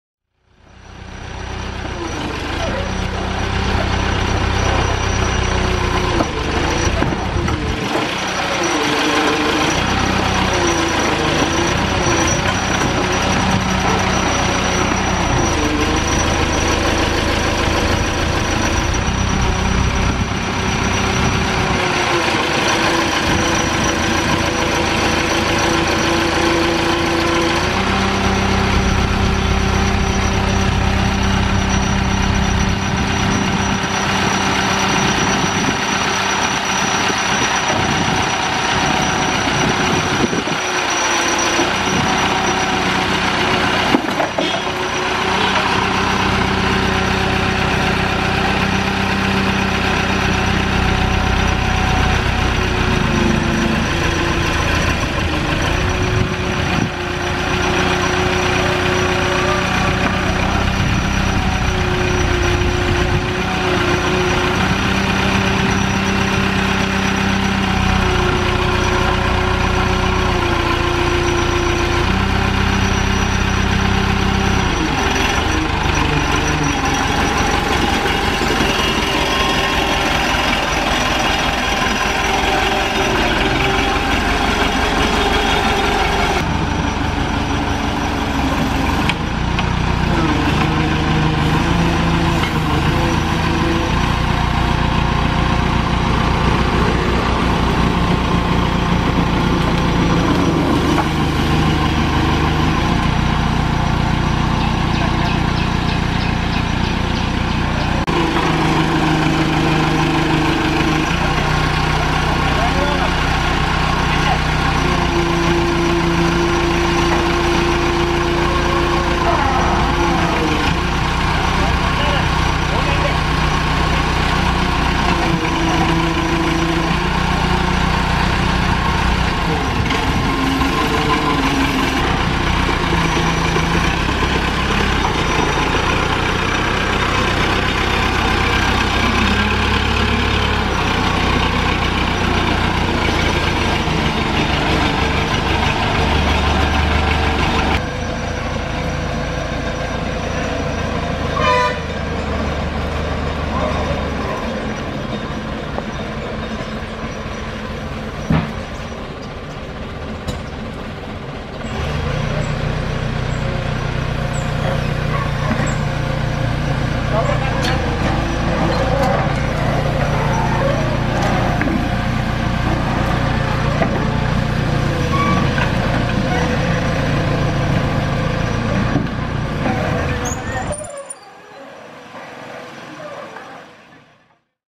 The Powerful Heavy Equipment JCB Backhoe Lifting Water Main Pipe Road Workers Without Safety
The powerful excavator machine lifting huge water main pipes on this road construction site bringing water to village. The construction workers are helping the excavator operator but without any safety actions.

METADATA1